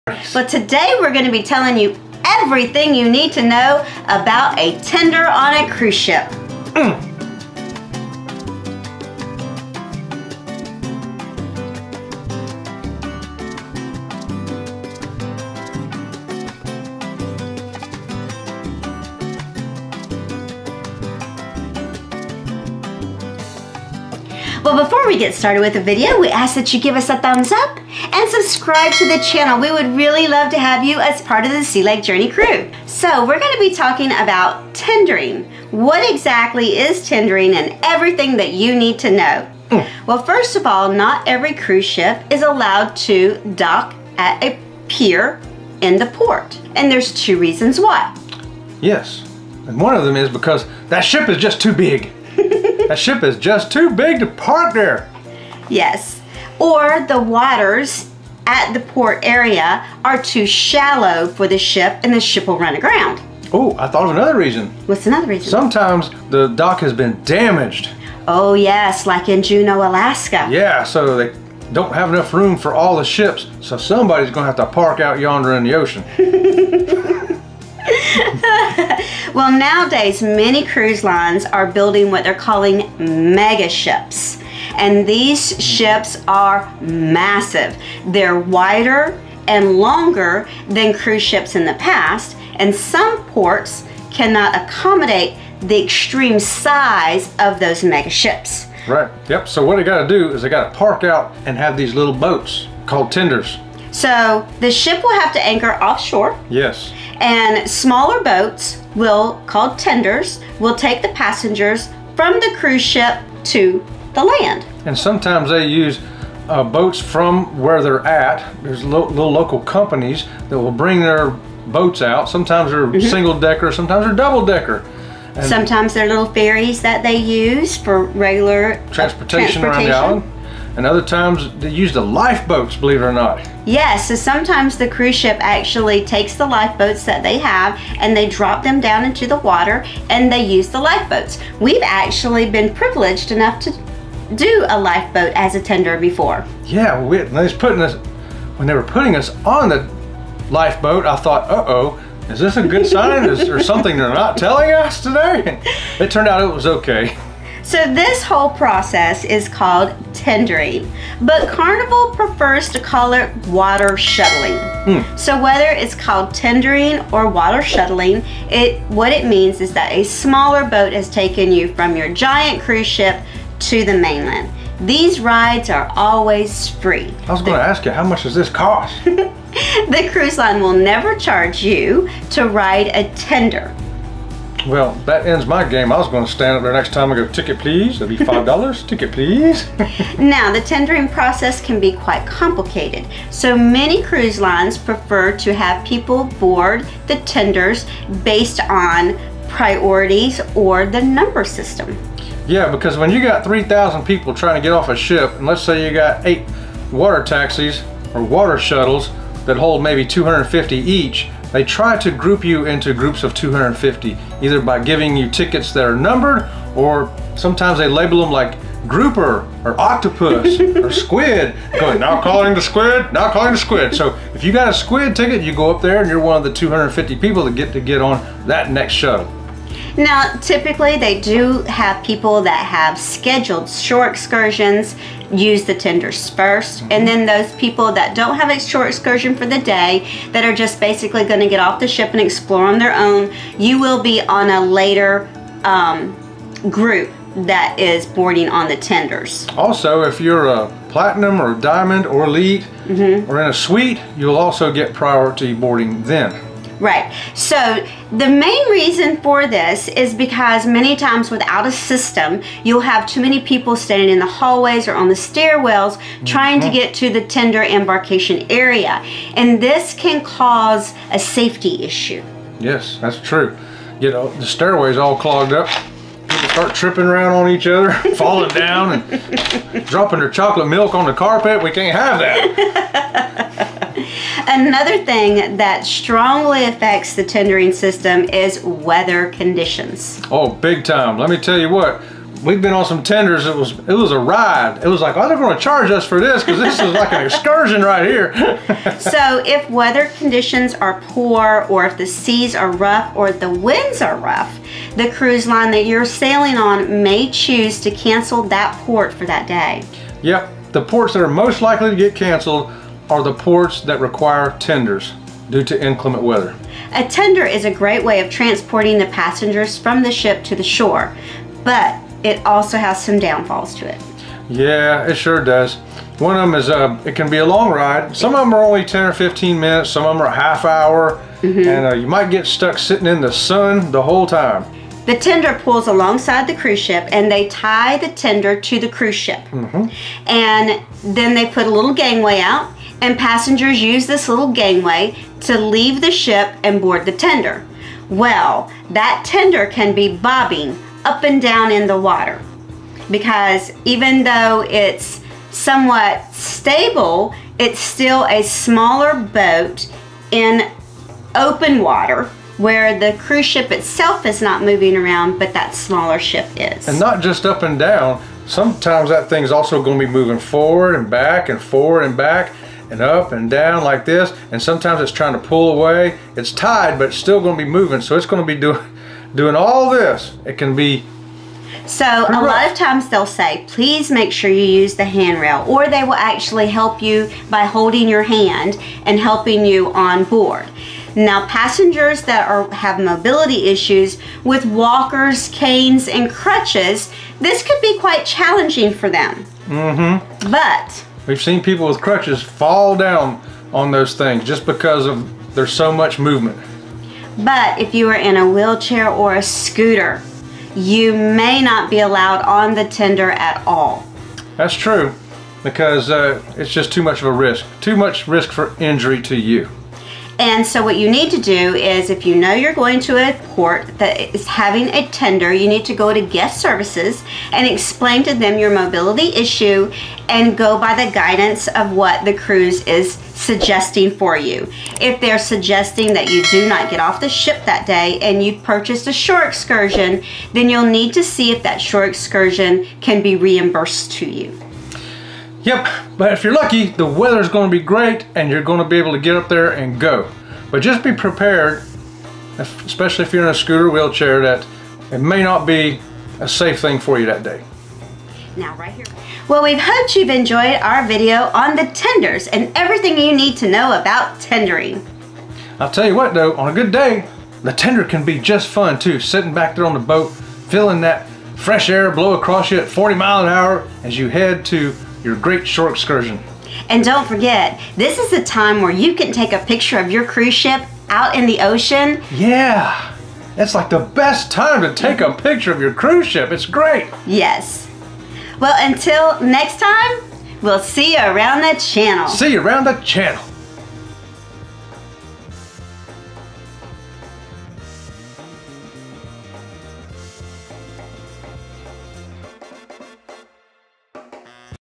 Tendering 101: Everything You Need to Know With Sea Leg Journeys
Ship tenders are boats used to transfer passengers from the cruise ship to the port they are visiting. Come with us and find out why tenders are used. We also have important information about tenders for those who have mobility issues.